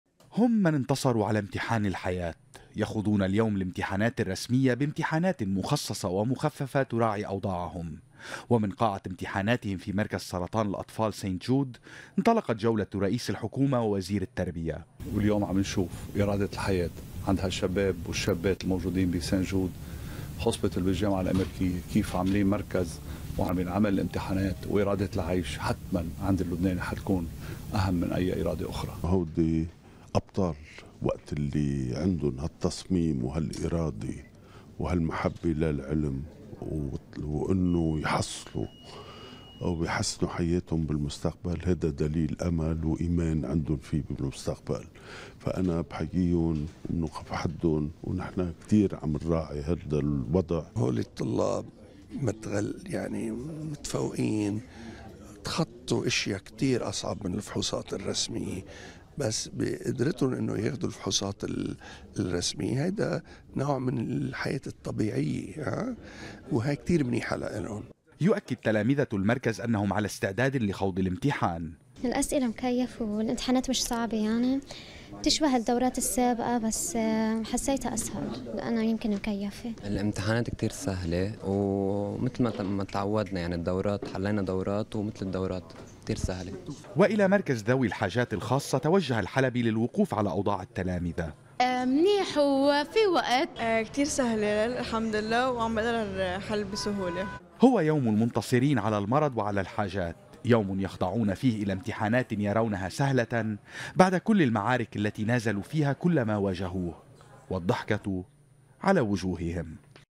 تلاميذ مركز سرطان الأطفال يَخوضونَ الامتحانات الرسمية
جولة لرئيسِ الحكومة ووزيرِ التربية على تلامذة مركز سرطان الاطفال الذين يَخوضونَ الامتحانات الرسمية، وميقاتي يؤكد ان الارادة تتجلى في شبان وشابات المركز.
تقرير حسان الرفاعي